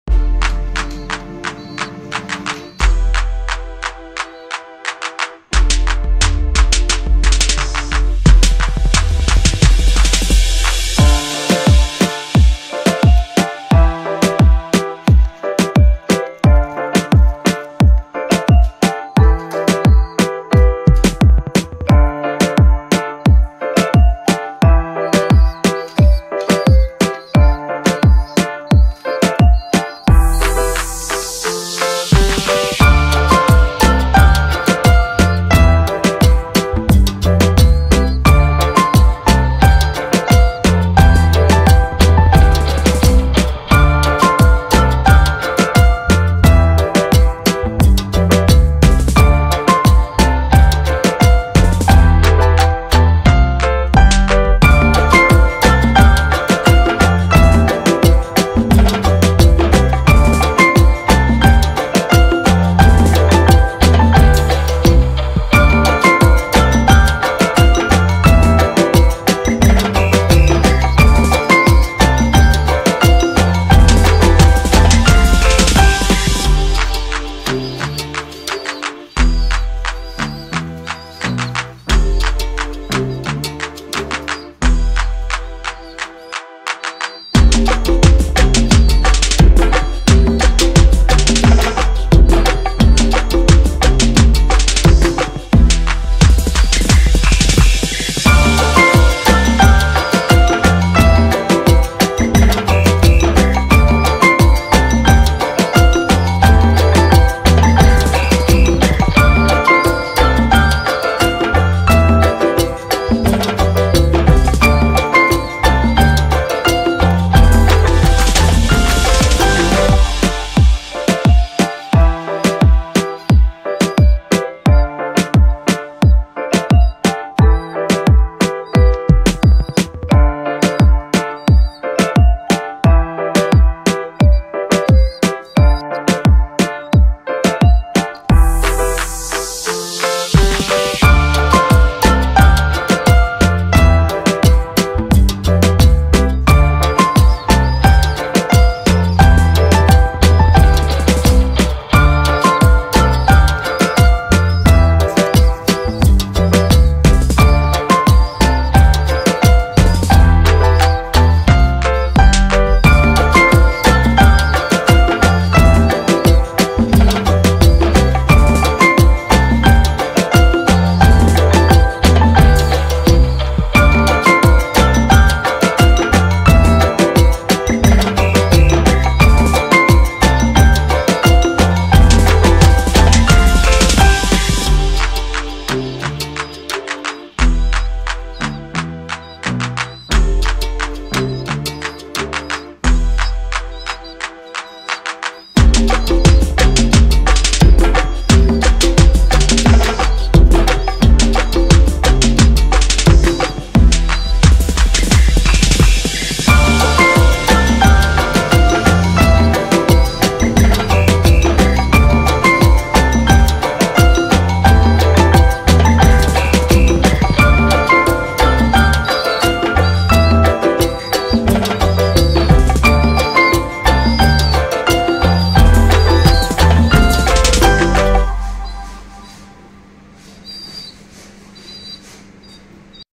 Kabotaron ky bachy Hand Feed Krna Start Hogy !
In This video:
Asslam Alaikum Dear viewers Kiya Haal hai umeed krty Hain ky ap Saab theek thaak Hoon gy. Saab sy phly apko btaty chalin ky yeh mere kabotaroj ky choty bachy thy jinko hand tame kr Raha hun mn.